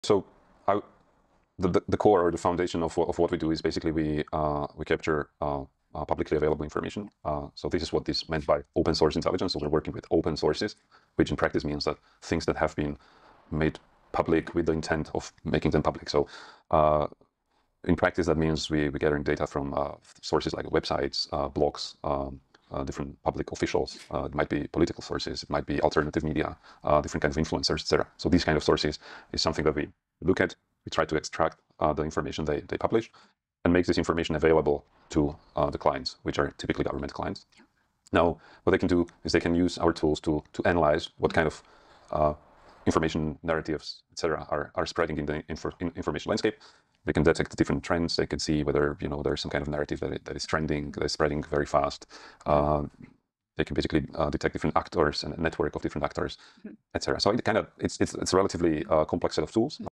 The Power of Open Source Intelligence
How Tech companies like Gerulata Technologies provide AI tools to combat disinformation and hostile propaganda